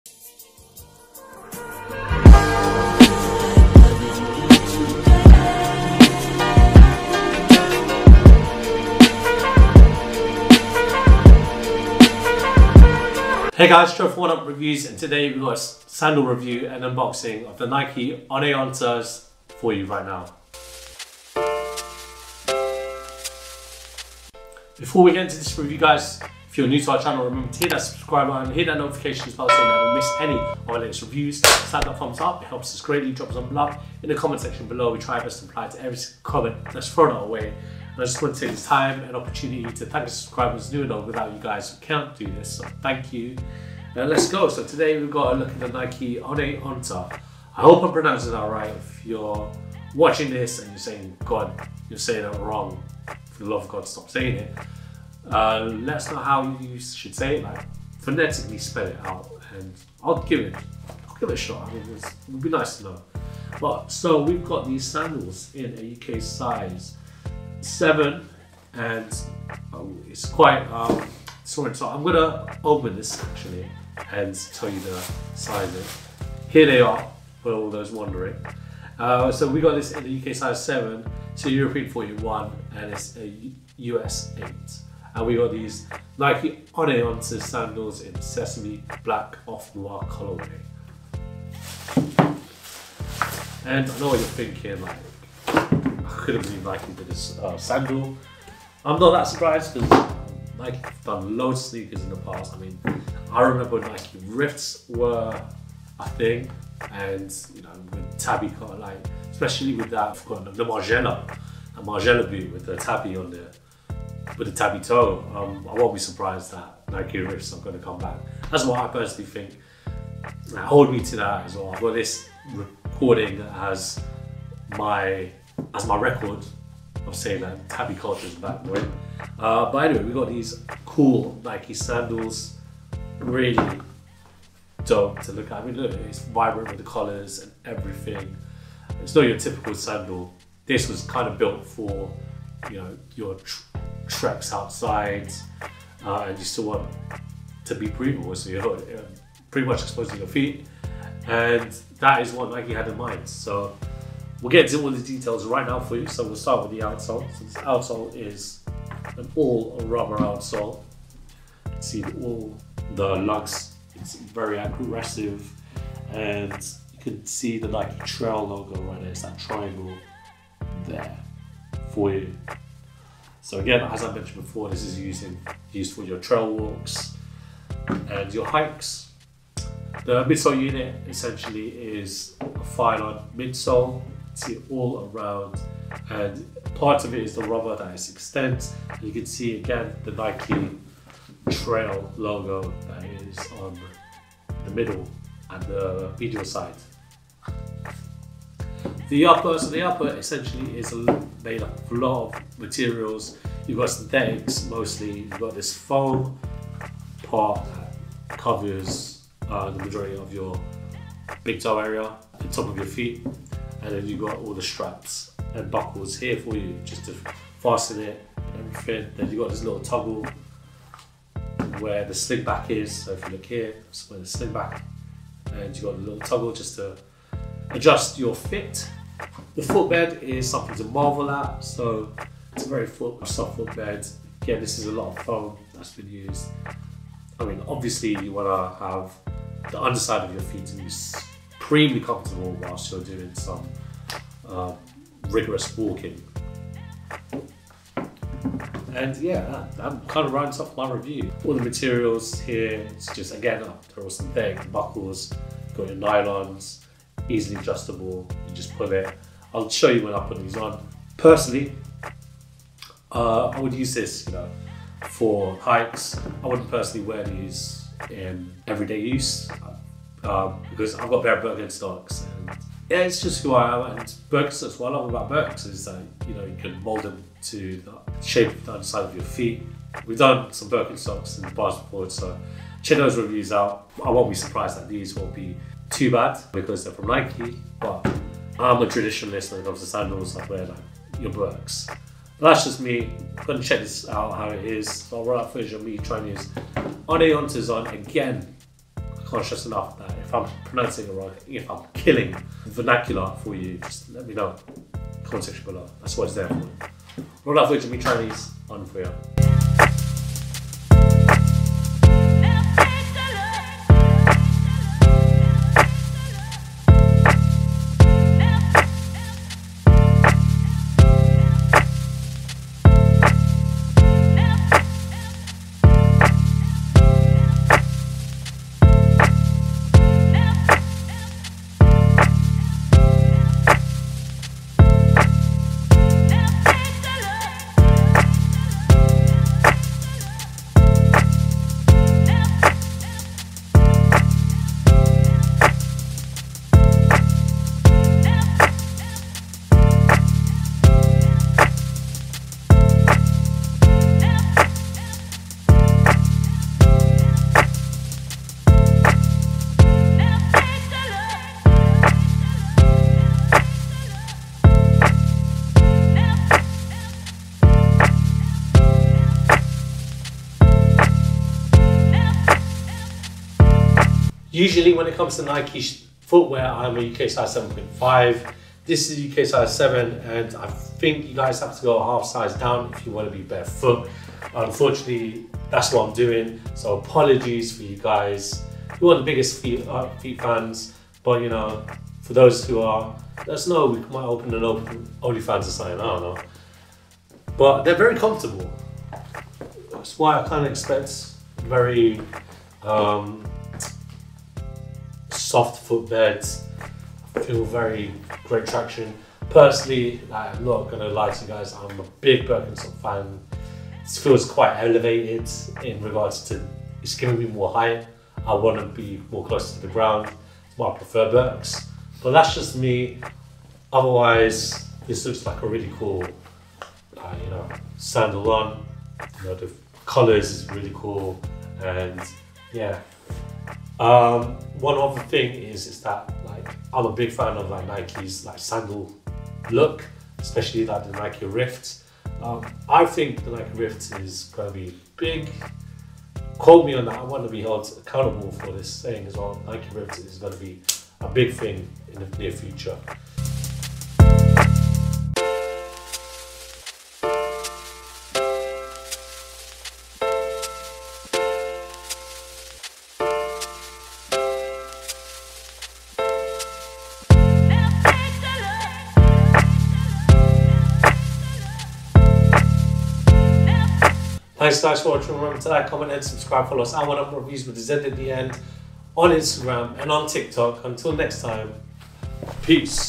NIKE ONEONTA NEXT NATURE SANDALS | Unboxing, review & on feet
Get true to size!

0:00 NIKE ONEONTA NEXT NATURE SANDALS
1:17 unboxing
5:10 180 degrees view
5:56 on feet
6:15 sizing